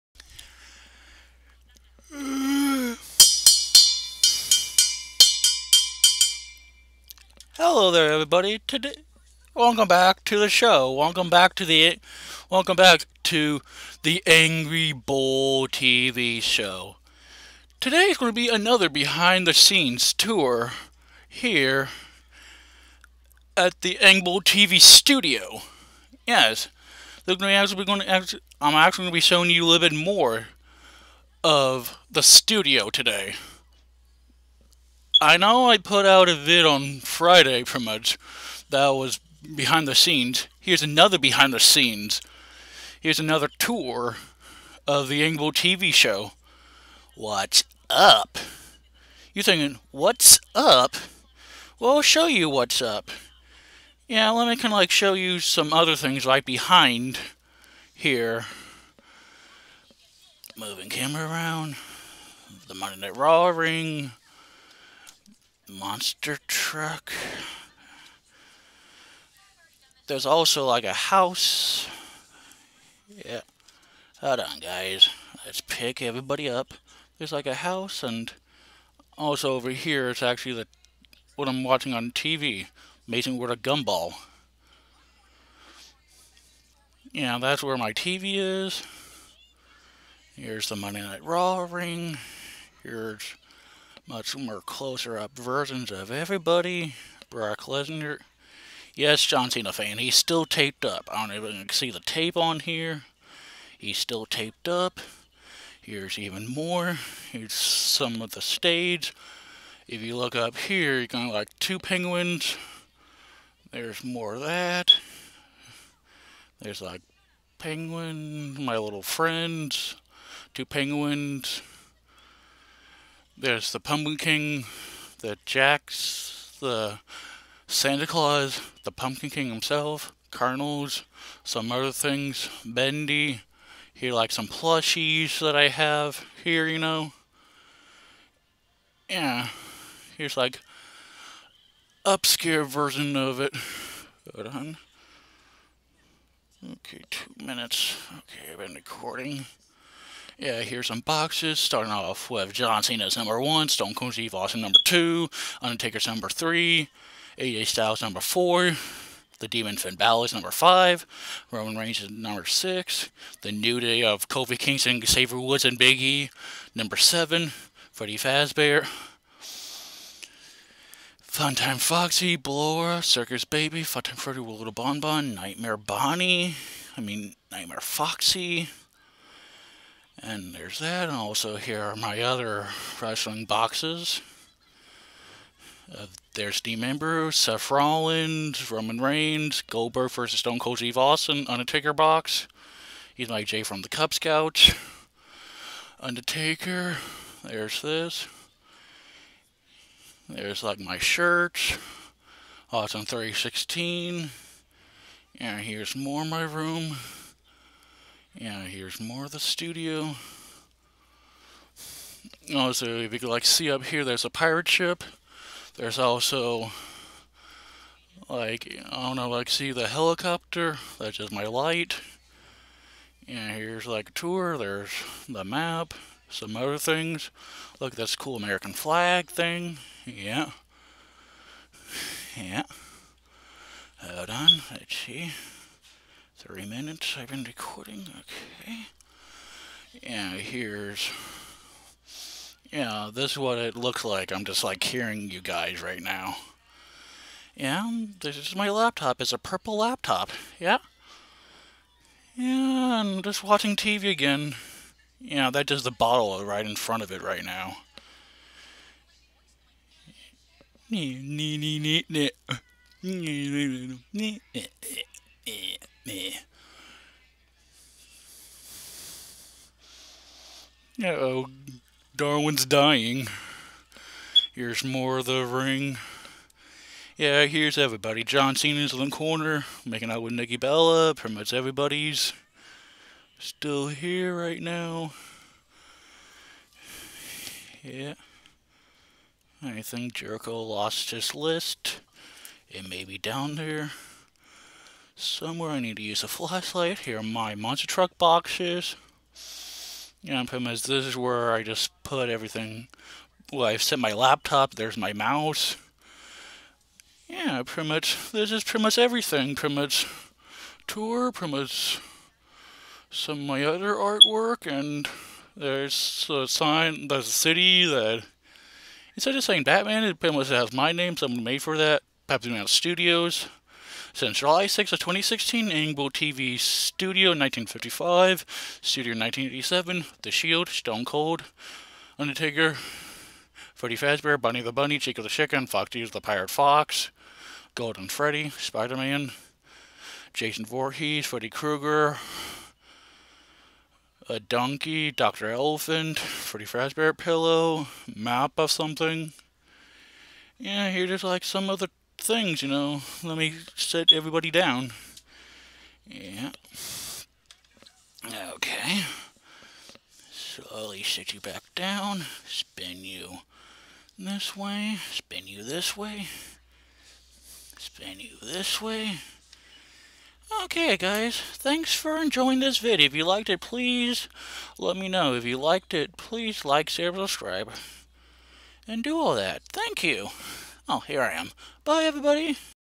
The ANGRY BULL TV Show behind the show part 2
here on The ANGRY BULL TV Show I let everybody the studio in where I make The ANGRY BULL TV Show and sorry if you get sick when I was flying with the laptop so sorry about that